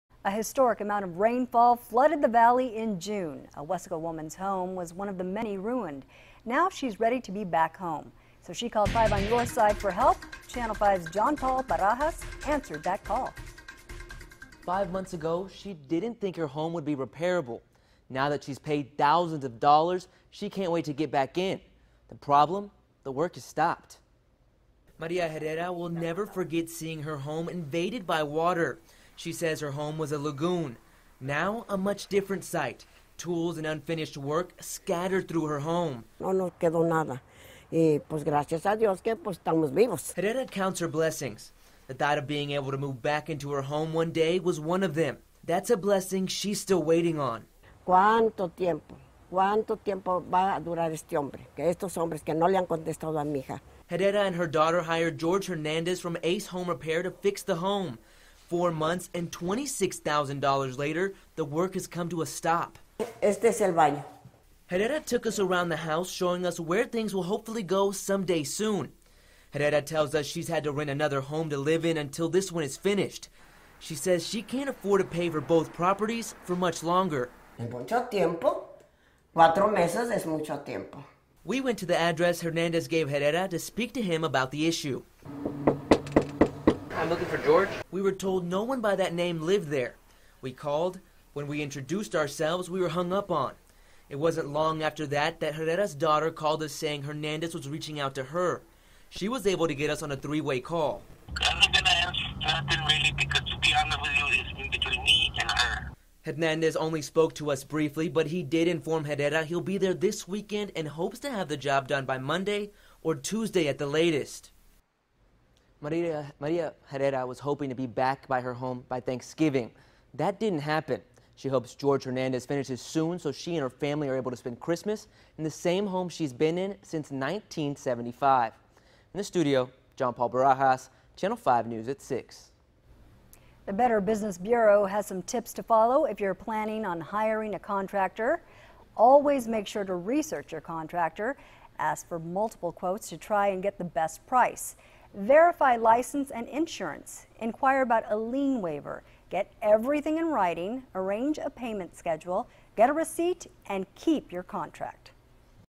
Homeowner Waiting on Contractor to Complete Repairs Months Later
Her Weslaco home sustained water damage during the June floods.